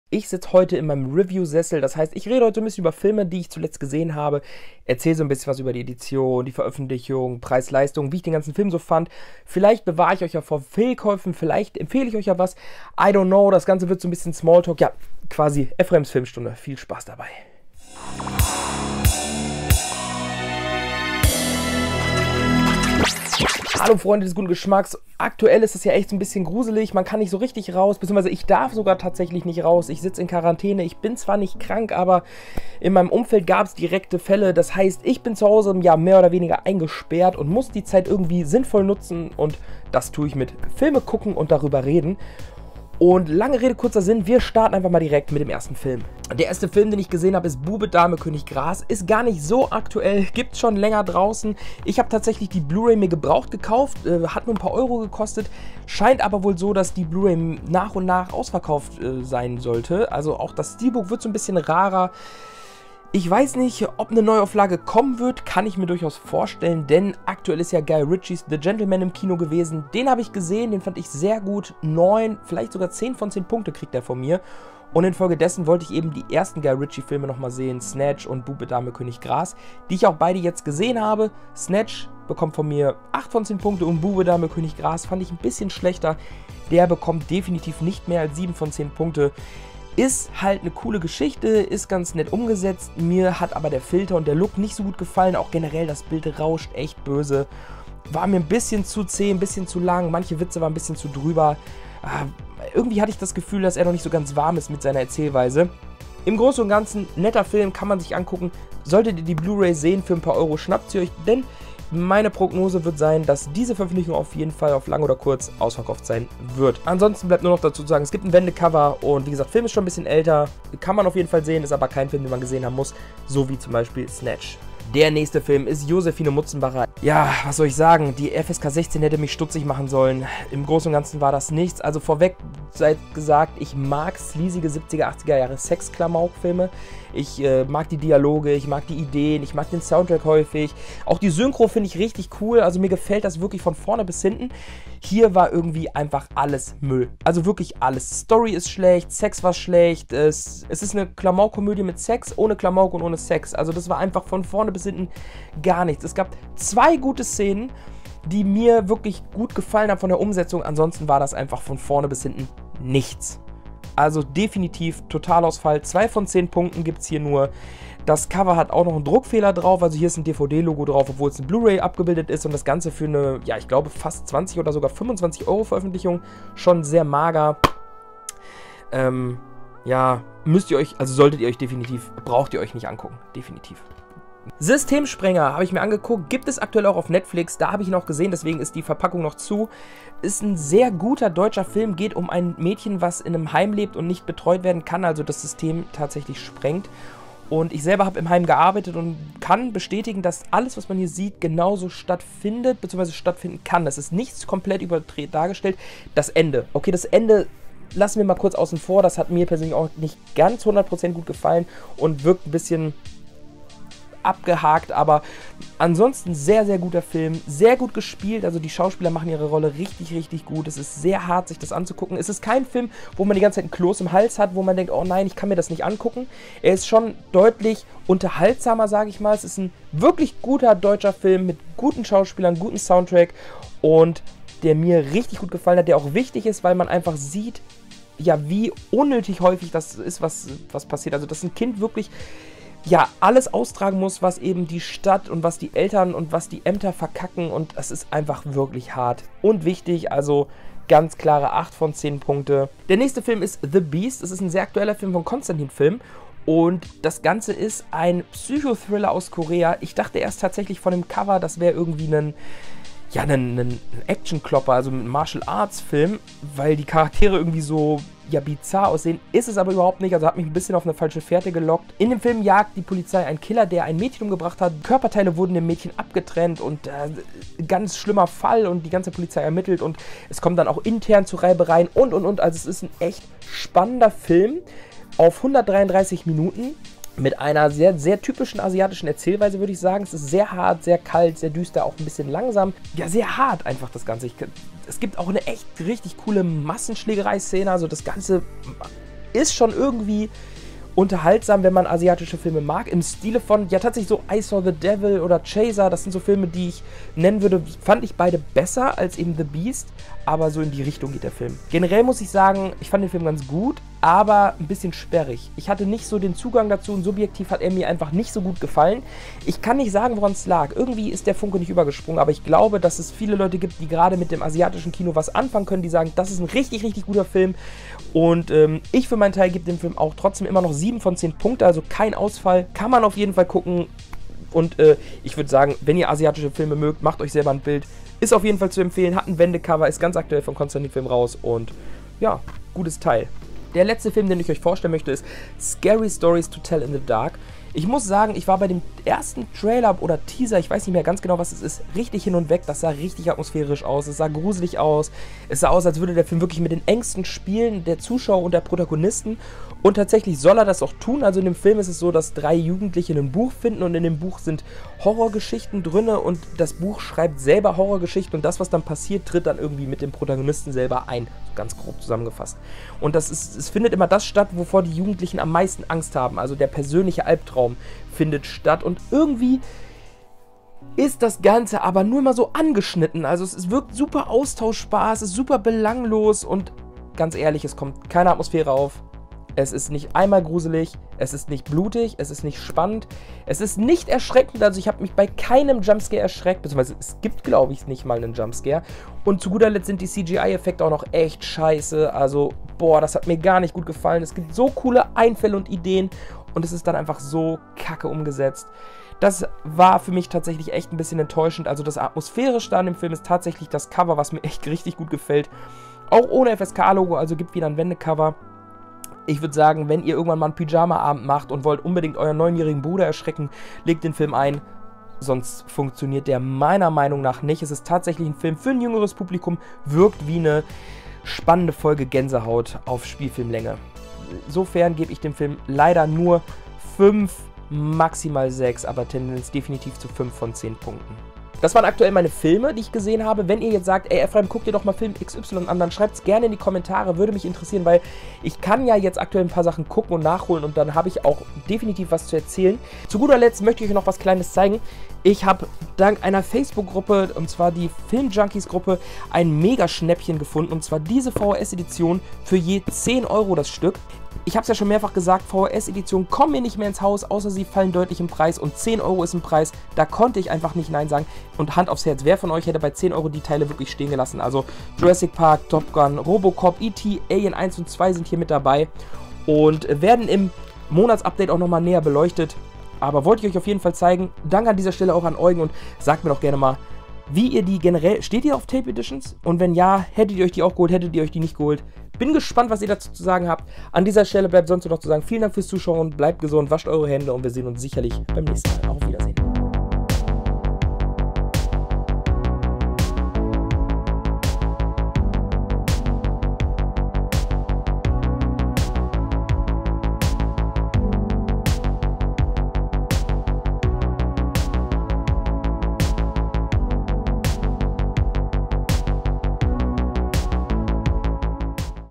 Quarantäne Woche 1: Das Filmtagebuch
Aktuell sitze ich zu Hause fest und dachte mir, ich erzähl mal etwas über meine letzten Einkäufe und kann euch somit evtl die ein oder andere Filmentscheidung abnehmen!

Die Filme kaufen? Hier ↴
Bube, Dame .. (Stream)
Systemsprenger DVD
Systemsprenger Blu-ray
The Beast DVD
The Beast Blu-ray
Scary stories... DVD
Scary stories... Blu-ray
(Ref-Links!)